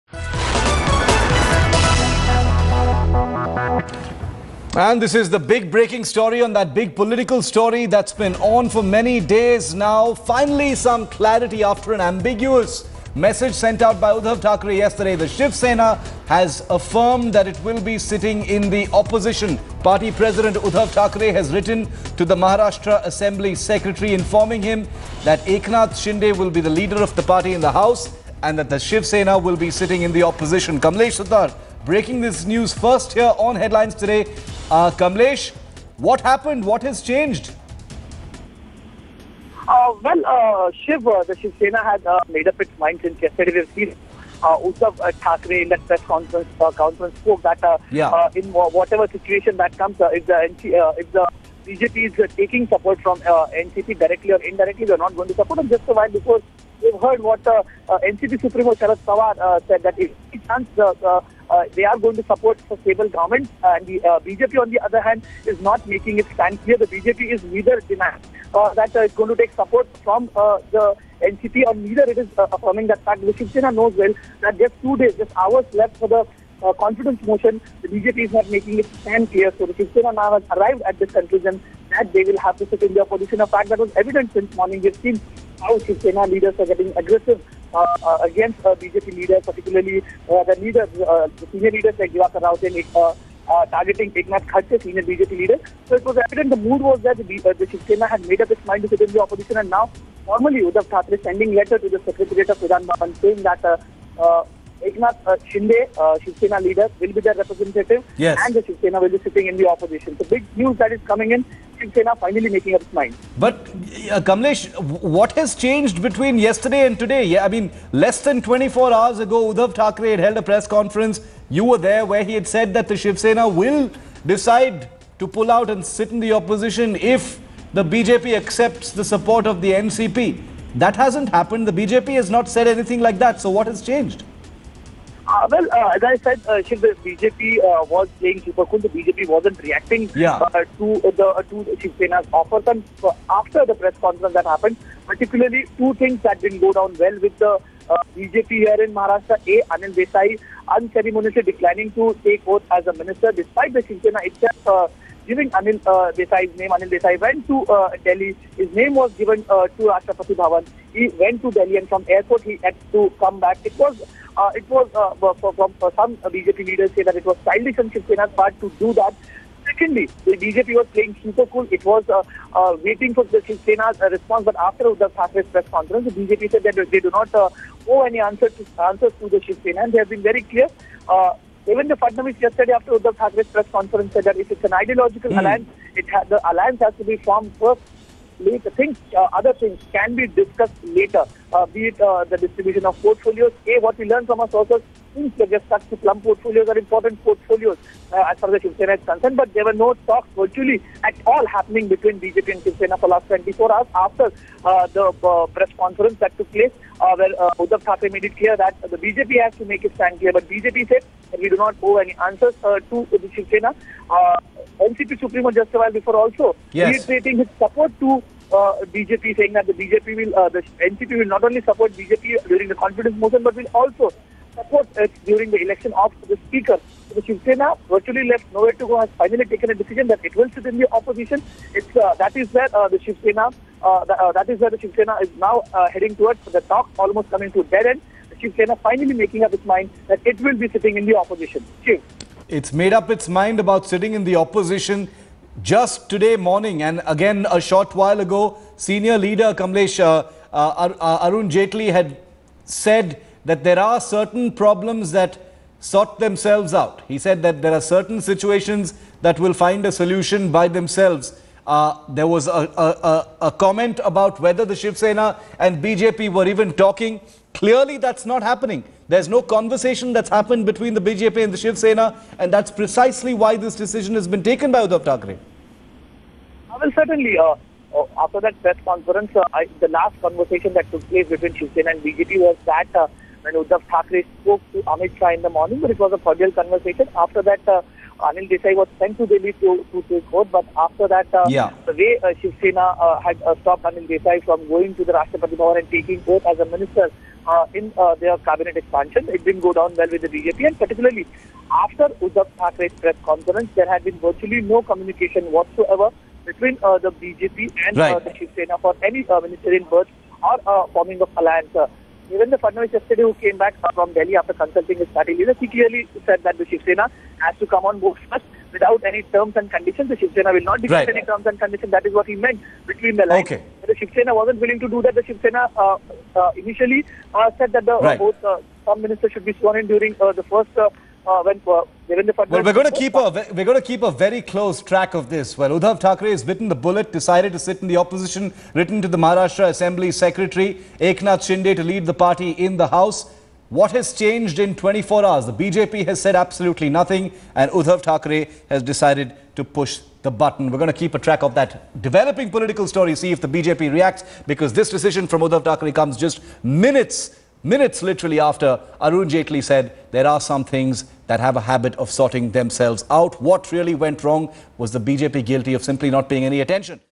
Confirmed: Shiv Sena To Sit in Opposition In Maharashtra Assembly
Uddhav Thackeray has confirmed that the Shiv Sena will sit in Opposition in the Maharashtra Assembly. The news broke after Thackeray wrote to the Maharashtra Assembly secretary confirming his party's place in the assembly. He added that Eknath Shinde will be their leader in the house.